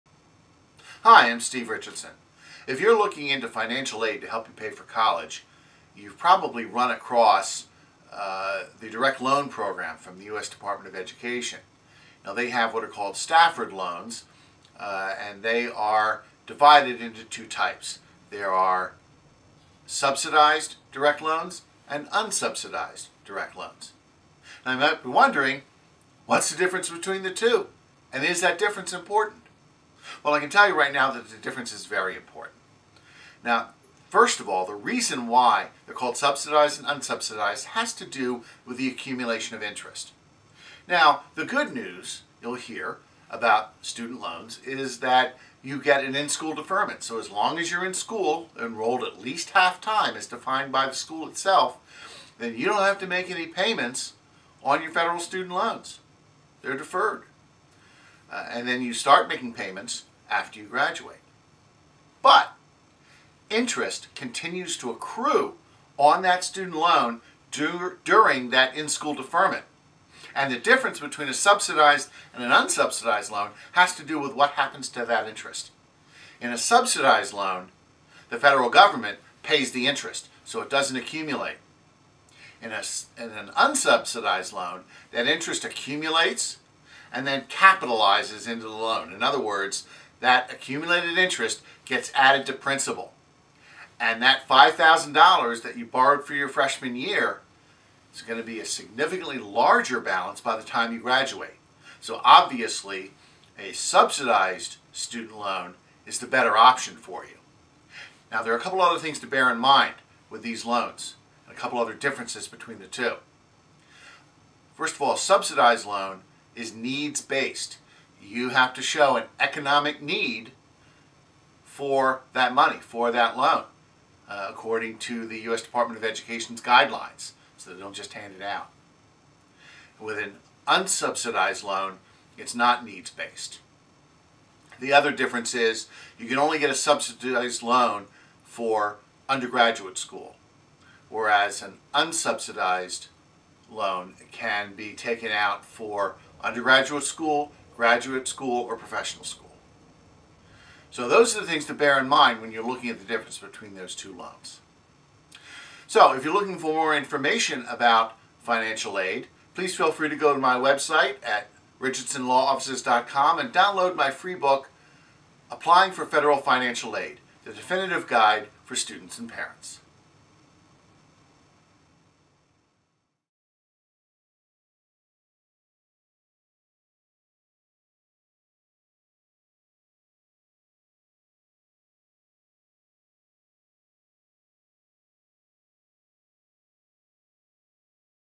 Subsidized v. Unsubsidized Student Loans; The Important Differences
The U.S. Department of Education’s Stafford student loan program has both subsidized and unsubsidized loans, and knowing the difference between the two is very important. These differences affect eligibility, use (undergraduate or graduate school), and interest accumulation.

Subsidized loans are available only to those in financial need. This means that the school determines the amount you can borrow, and it cannot exceed your need.

In general, financial need is the difference between the cost of attendance and your Expected Family Contribution. With unsubsidized loans, you do not have to demonstrate need.

Use. Subsidized loans are only available for undergraduate school, while unsubsidized loans can be used by graduate students as well.

This is where the names of the two loan types comes into play. With subsidized loans, the U.S. Department of Education pays the interest during the following periods, while with unsubsidized loans, you do:

*   While you are in school at least half time
*   For the first six months after you leave the school or graduate (the grace period)
*   During a period of deferment of loan payments after you leave/graduate from the school

With an unsubsidized loan, the interest accumulates during these periods, and then capitalizes (is added to the loan principal) after the period is over.

As for the grace period, the interest that accumulates during the grace period on a loan that disbursed between July 1, 2012, and July 1, 2014, must be paid or it will capitalize.